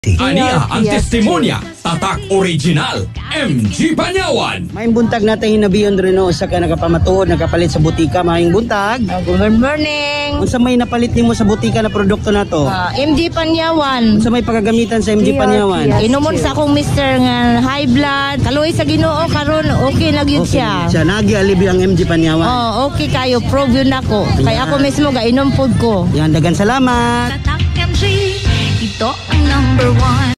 MG Panyawan Capsule Testimonial Radio Ad 2020-2024 30s (Version 7) [ST]
Ito po ang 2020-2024 Radio Ad ng MG Panyawan Capsule  (Version 7).

DRPSTwo 8907: MG Panyawan Capsule Testimonial Radio Ad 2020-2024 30s (Version 7) [ST]